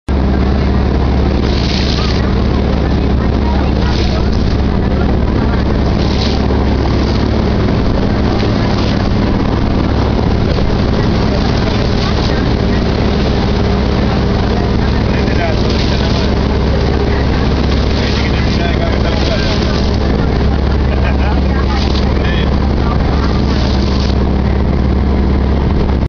Navegando por el rio Sarmiento 1
Navegando en lancha turistica en el Delta del Tigre , Argentina